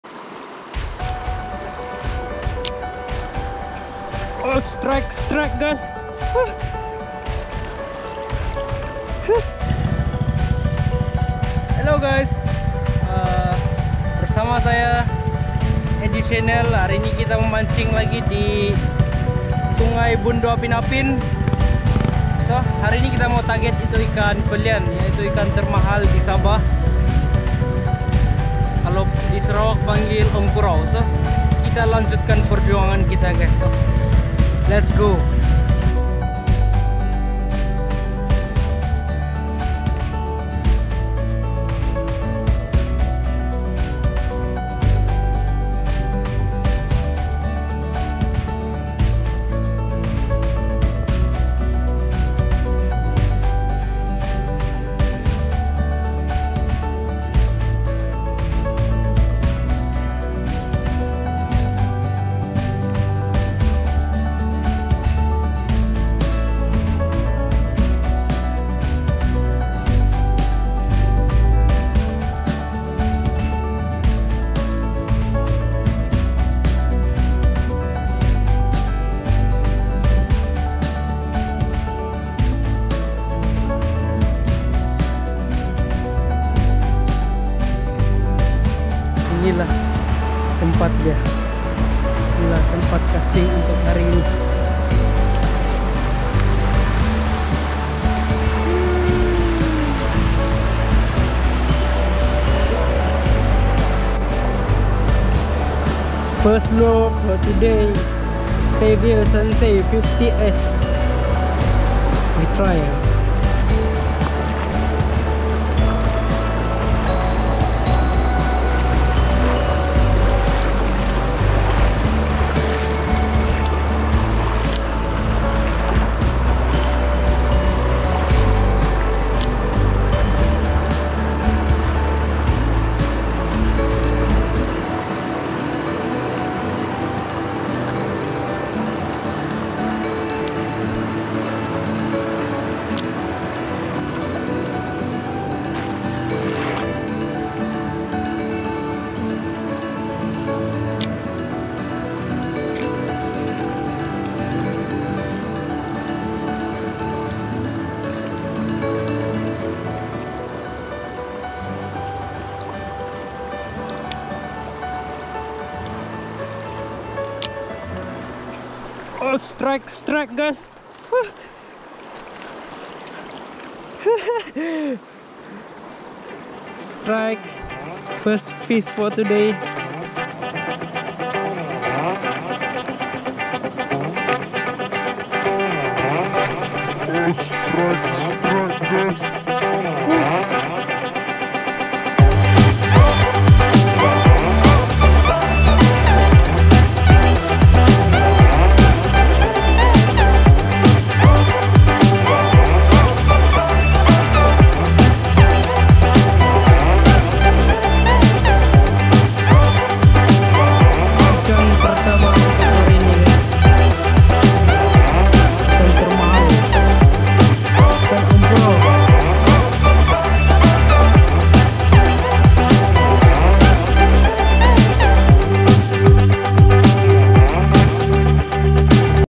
MANCING IKAN KELAH / PELIAN SABAH RM25 1KG.
Memancing dalam beberapa minit sebab cuaca tak mengizinkan so see u guys next strike for kelah sabah.